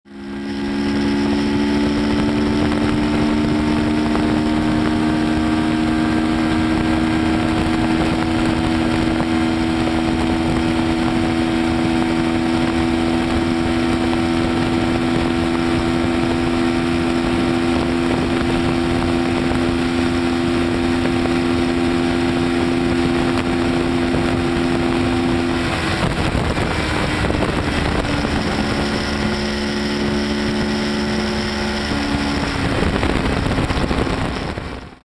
2010 Kawasaki Ninja 650R Top Speed 220 km/hr
2010 Kawasaki Ninja 650R top speed run. This is about as fast as she'll go. She's stock except for an Arrow exhaust, airbox mod and pair valve plugged.
Curious to see if I'll get a bit more once the Powercommander V and Autotune are installed. (Oh, and sorry about the terrible video quality...)
Ride On!